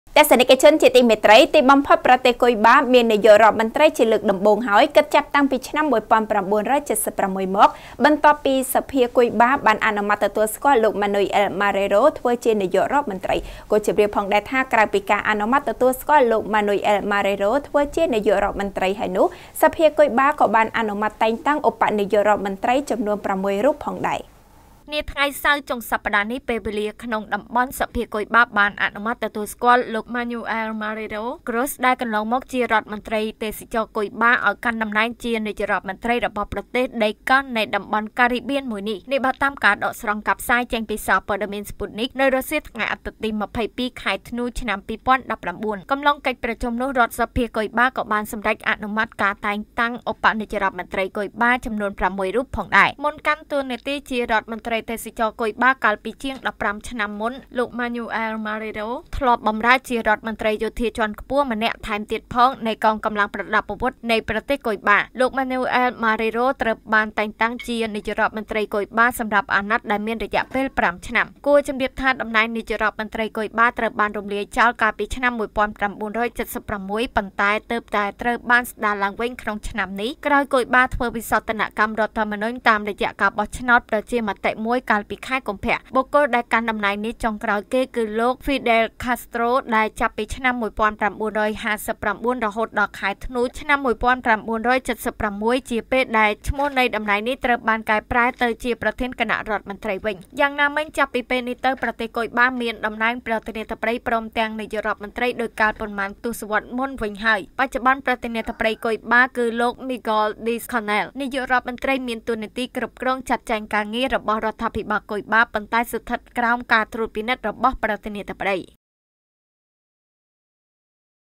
ទីបំផុតប្រទេសគុយបាមាន «នាយករដ្ឋមន្រ្តី» ឡើងវិញជាលើកដំបូង.
ទីបំផុតប្រទេសគុយបាមាន «នាយករដ្ឋមន្រ្តី» ឡើងវិញជាលើកដំបូង គិតចាប់តាំងតែពីឆ្នាំ១៩៧៦មក
* រាយការណ៍ព័ត៌មានដោយកញ្ញា យ៉ង់ សក្គុណា  និងអ្នកនាង កេត សត្យា, ទូរទស្សន៍ព័ត៌មាន Fresh News